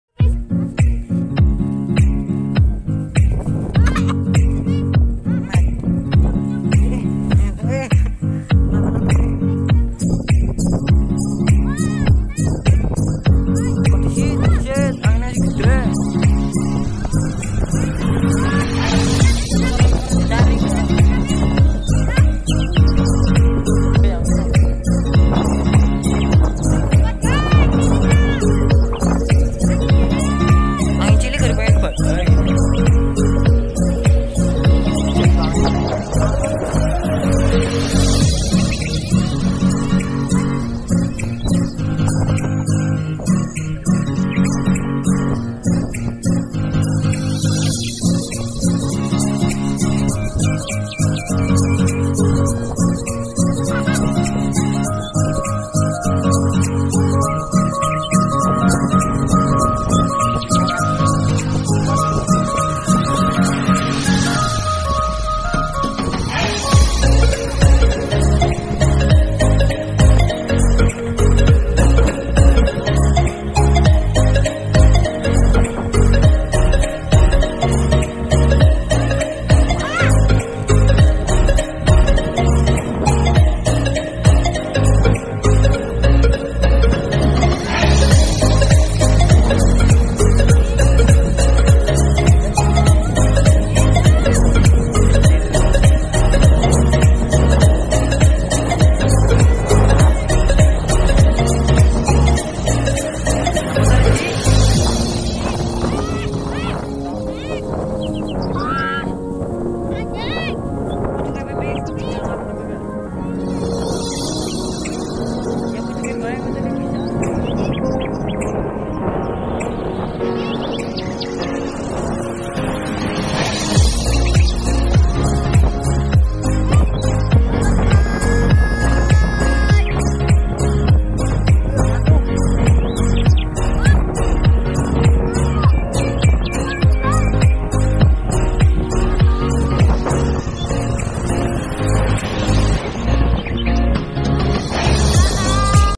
Layangan BEBEAN buatan bocil keren terbang
layangan bebean asli buatan bocil unik bisa terbang bukan kaleng kaleng